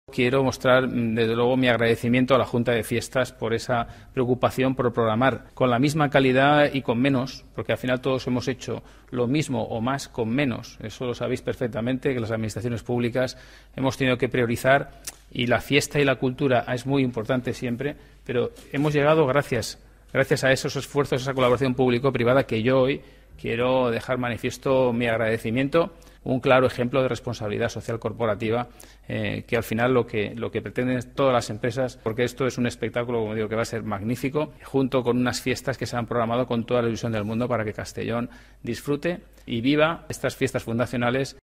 La música de una ‘All Star Band’ el 'Conciertazo' de las Fiestas de la Magdalena
El Alcalde de Castellón, Alfonso Bataller, ha presentado hoy el Conciertazo Amstel Magdalena que se celebrará el 12 de marzo en el Recinto Ferial de Conciertos y en el que actuará la ‘All Star Band’ de la que forman parte Daniel Gómez (guitarrista y director musical), Ronnie Romero (voz), Carlos García (batería), Juan Giner (teclados) y Javi García (bajo), que son la base del espectáculo y que contará con los artistas invitados Aurora Beltrán (Tahures Zurdos), Carlos Escobedo (Sôber), Sherpa (Barón Rojo), Pau (Uzzhuaïa), Ruth Lorenzo, Fortu (Obús) y el ganador del concurso televisivo ‘La Voz’ Rafa Blas que interpretarán los mejores temas de la historia del rock.